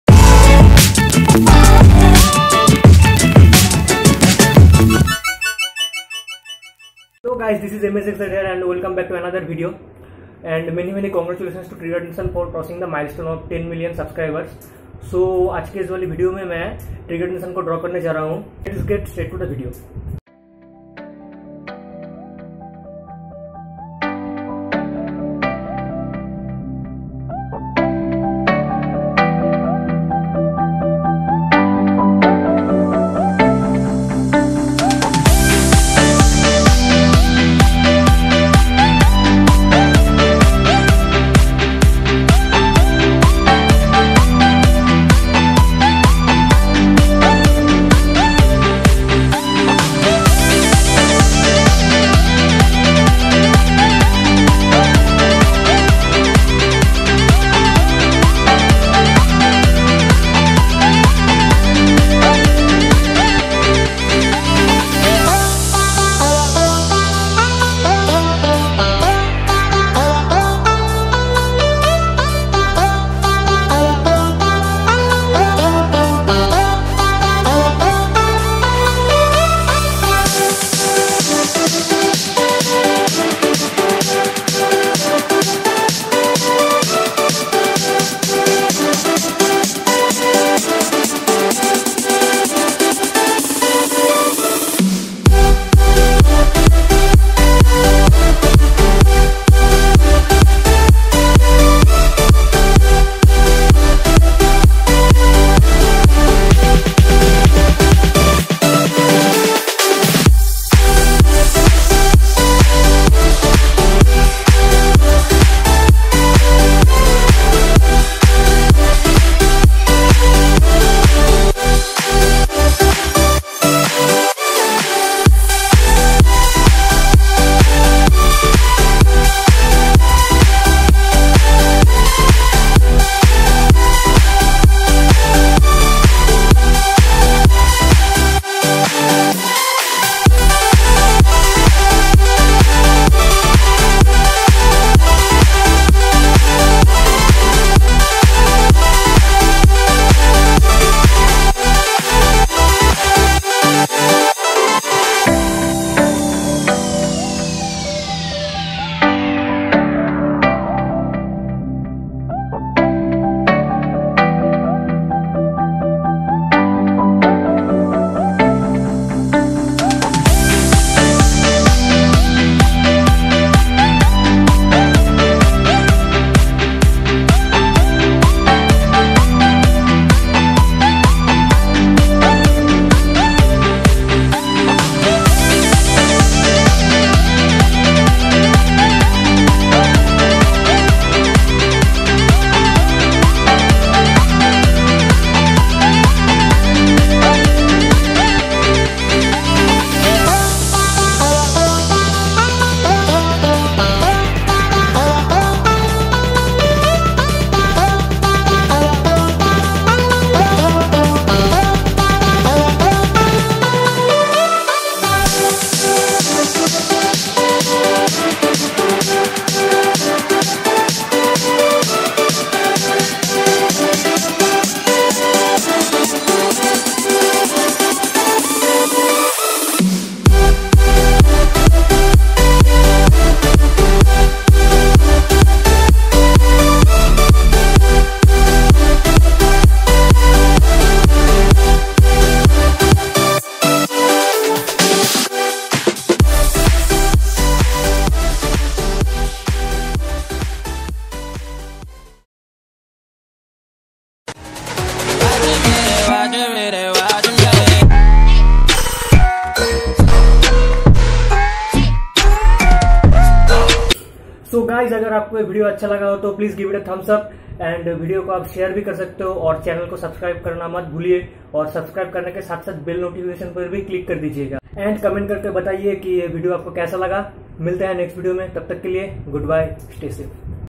Triggered Insaan | Drawing | MSxArt
Hey guys!!
Here is my new video. I drew @Triggered Insaan and many many congratulations to him for crossing 10M.

Hope you guys'll like it:-)

Materials I used:
1. Mechanical Pencil
2. Dom's Colour Pencil ( The cheapest colour pencil ever)
3. Soft Charcoal Pencil
4. Permanent Markers
5. Birstol Paper